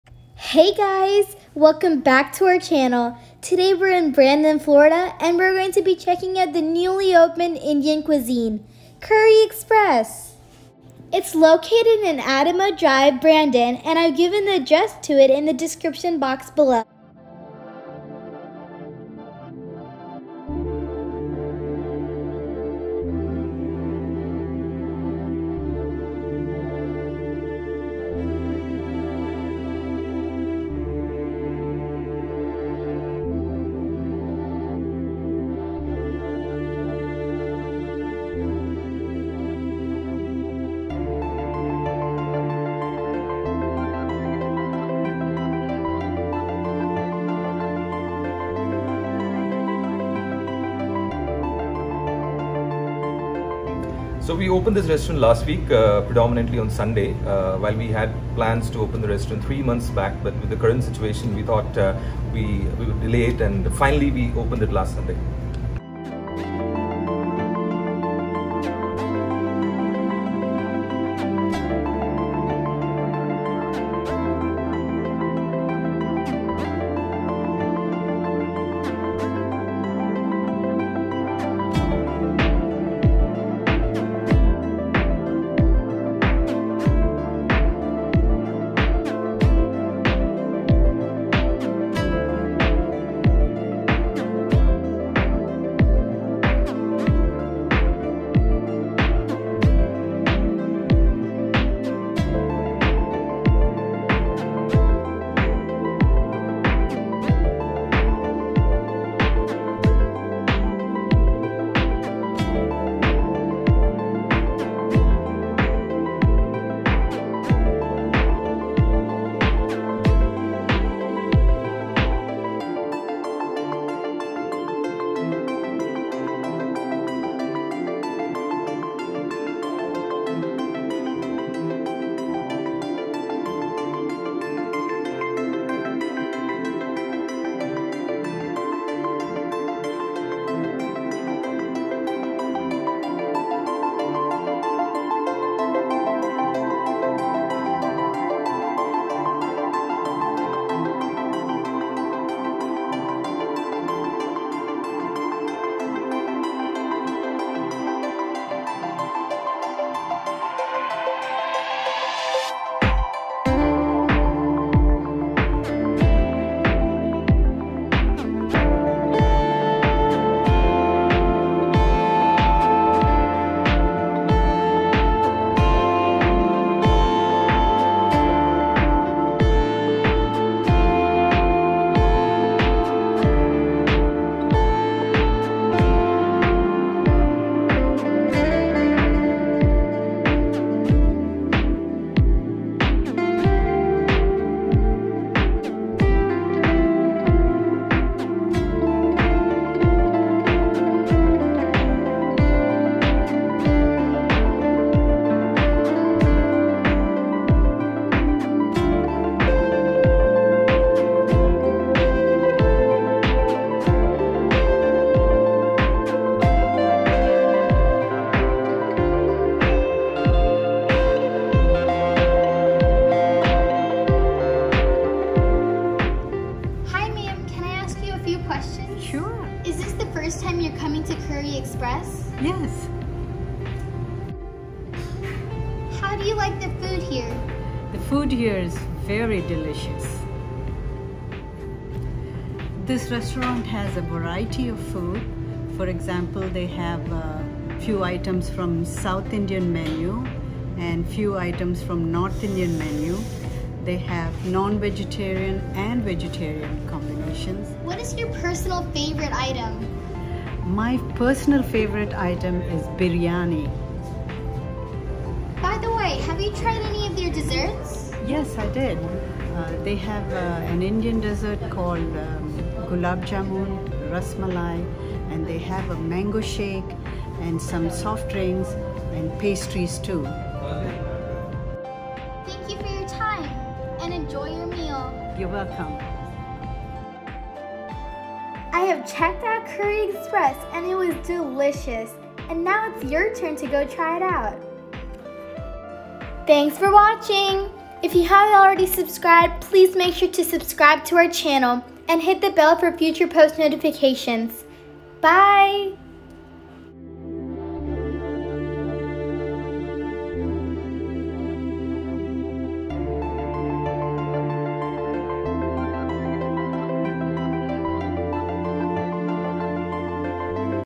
Kurry Xpress | New Indian Restaurant in Tampa | USA
Kurry Xpress is a newly opened Indian Cuisine in Tampa, FL USA. Kurry Xpress is located in Tampa near the Floor Decor and Old Time Pottery.
Address:
Kurry XPRESS
Brandon Crossings Plaza
Adjacent to Floor N Decor